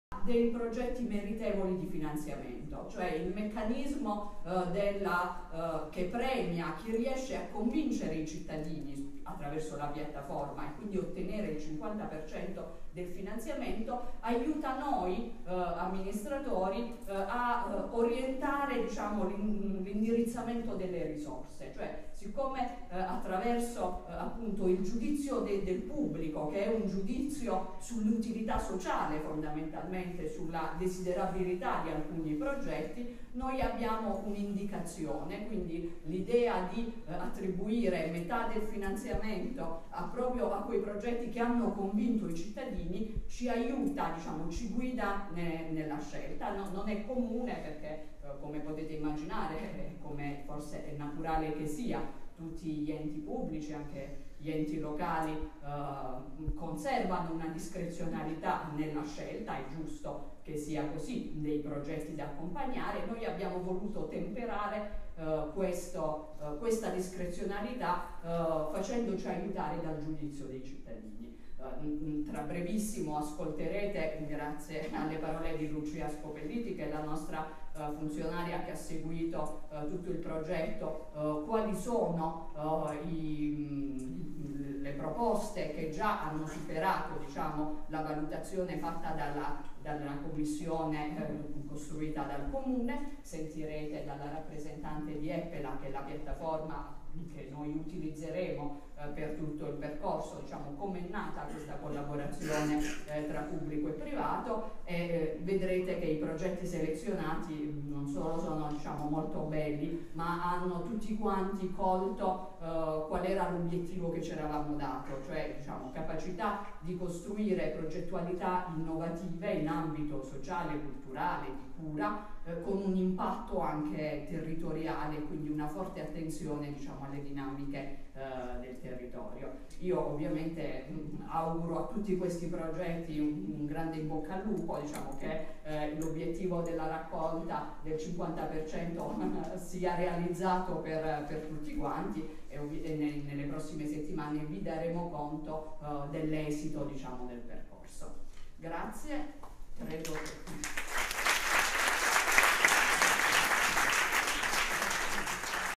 Crowfunding civico
Tajani e Majorino: “Costruiremo insieme ai cittadini la Milano del futuro, nel segno dell'innovazione e dell'inclusione ”